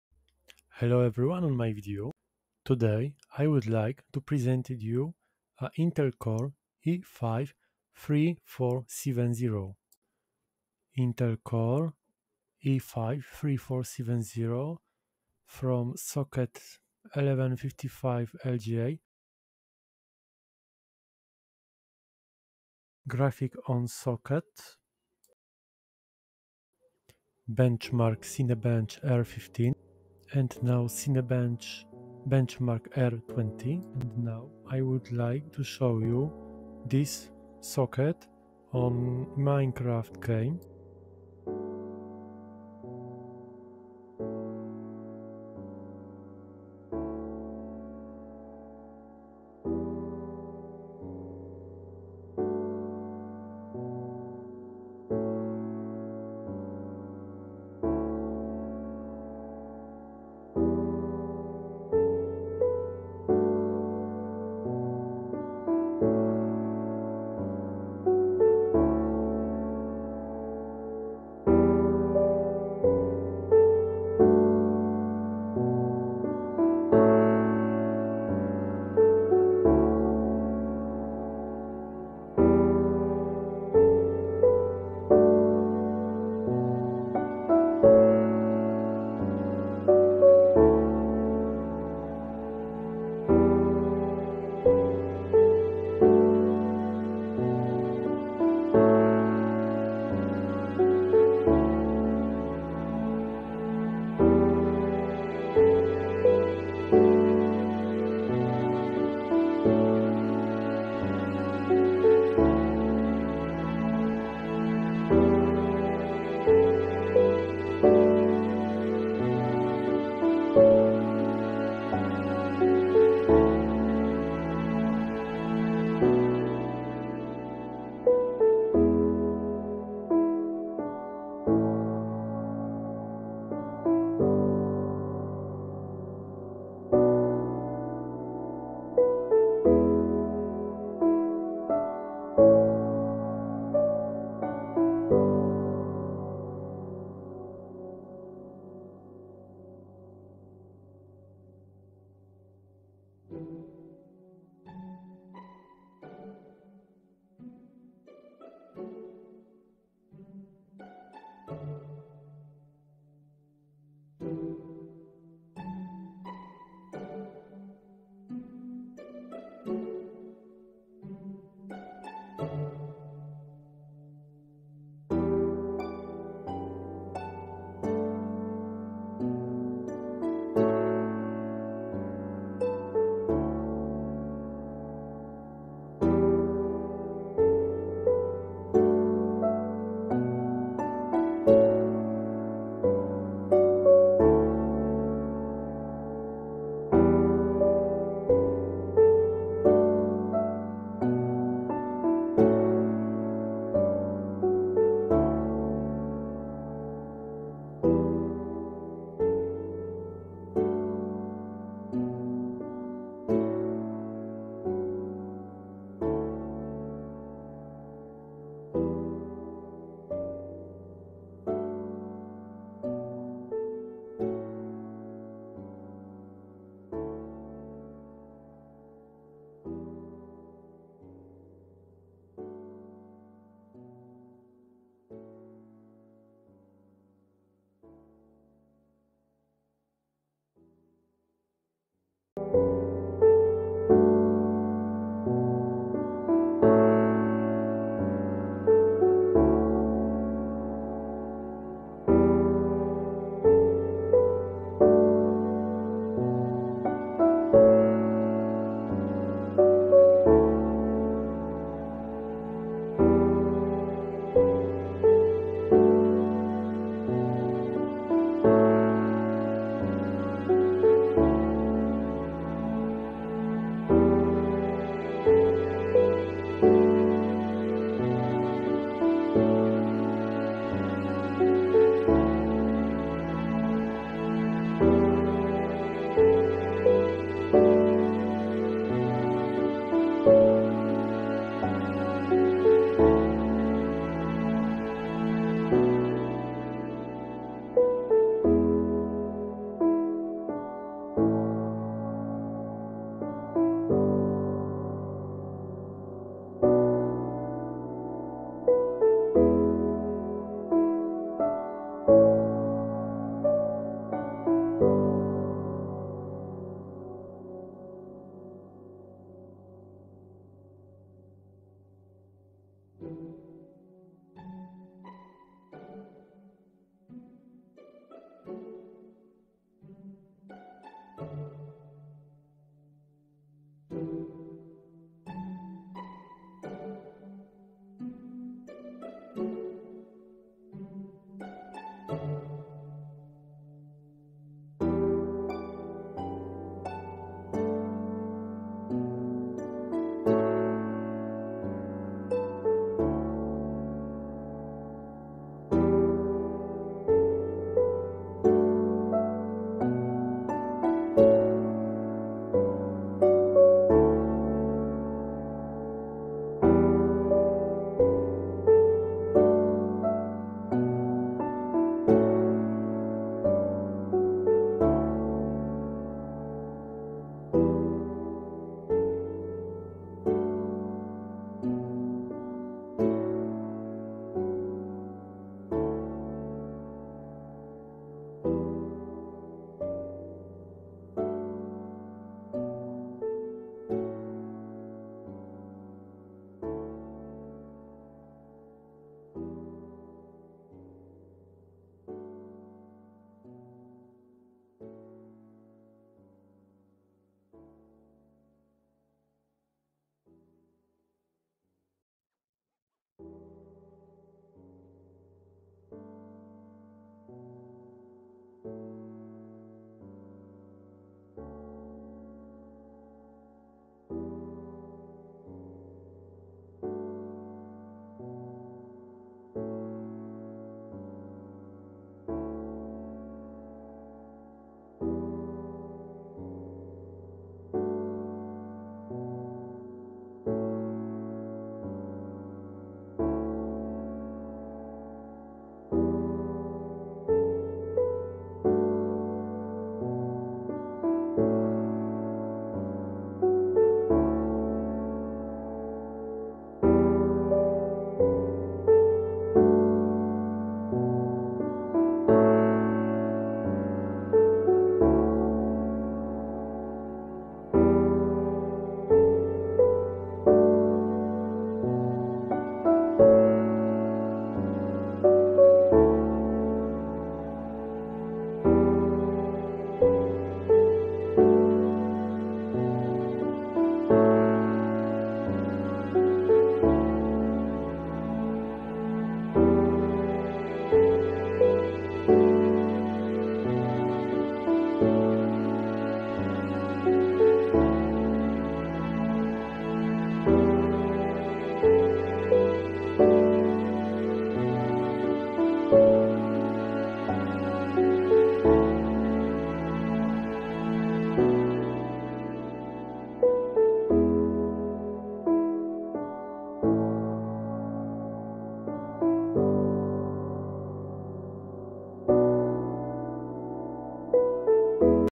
Intel Core i5 3470 | best socket ever?
▶ Hello everyone I would like to presented you a true power of i5 3470 with integrated graphics, no
     graphic card on this video.

▶ Specification technics: ✔️
▶ Name: Intel Core i5 3470 Ivy Bridge ✔️
▶ Socket: FCLGA 1155 ✔️
▶ Cores: 4 / Threads: 4 ✔️
▶ Basic Power: 3,2 GHz / Turbo Power: 3,6 GHz ✔️

▶ Cache: 6 MB ✔️
▶ Bus Speed: 5 GT/s ✔️
▶ TDP: 77 W ✔️
▶ Max Memory: 32 GB ✔️
▶ Type DDR: Ddr3 1333mhz ✔️ / DDR3 1600MHz ✔️
▶ Graphic: Intel® HD Graphics 2500 ✔️
▶ Graphics basic Power: 0,650 MHz / Turbo Power: 1,10 MHz ✔️